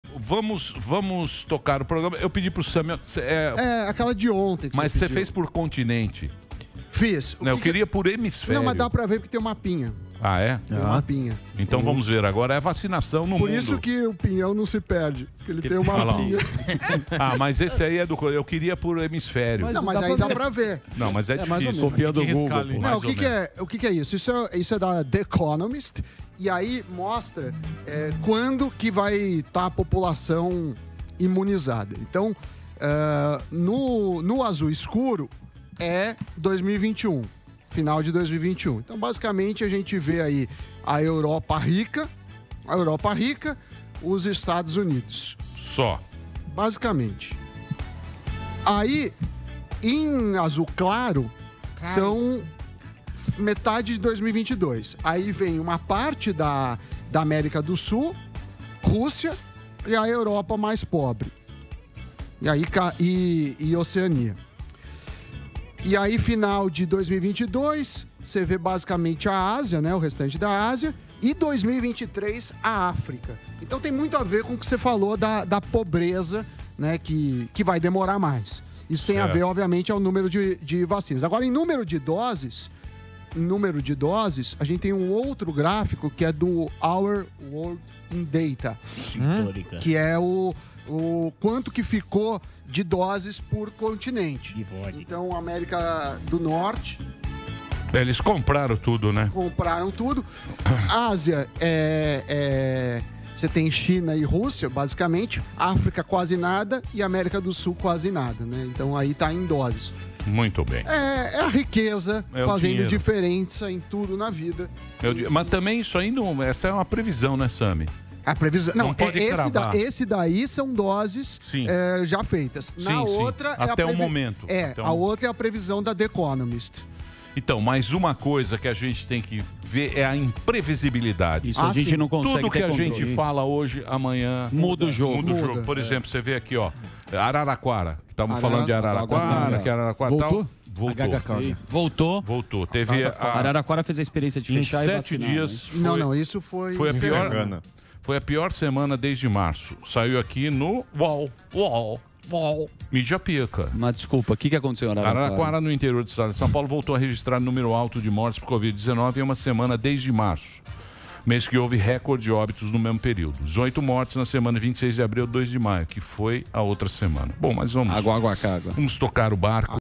VACINAÇÃO NO BRASIL SAI ANTES DA COPA DO CATAR EM 2022? SAMY DANA ANALISA
Samy Dana analisa o gráfico da revista The Economist que traz uma previsão de quando os países do mundo serão vacinados, e compara as estimativas dos EUA serem totalmente imunizados até o fim de 2021, e o Brasil até a metade de 2022